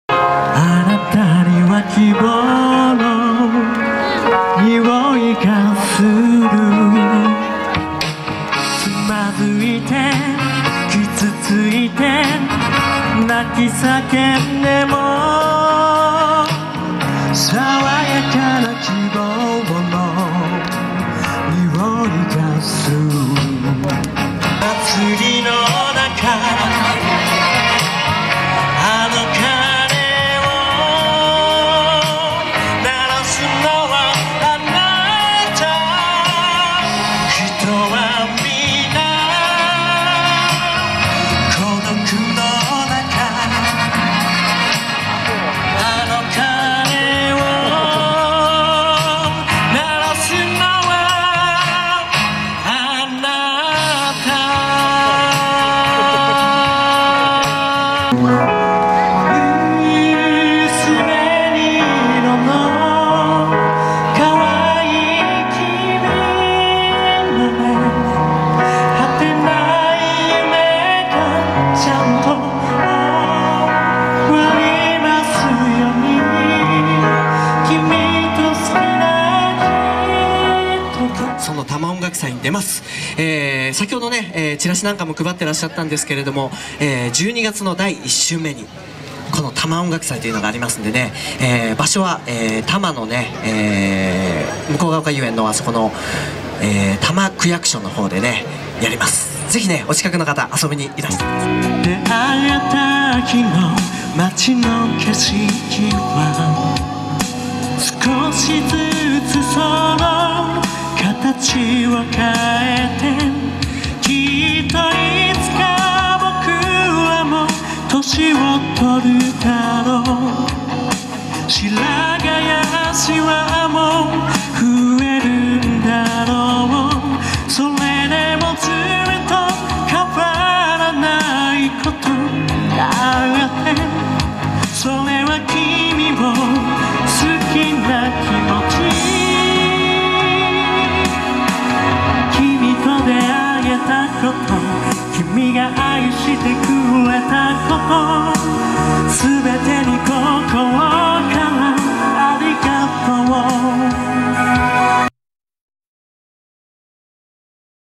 haru-T@ 第２回たま音楽祭バンドステージ出演者紹介
2012/12/1開催（多摩区総合庁舎）の第２回たま音楽祭、バンドステージに出演の
haru-T@の紹介動画。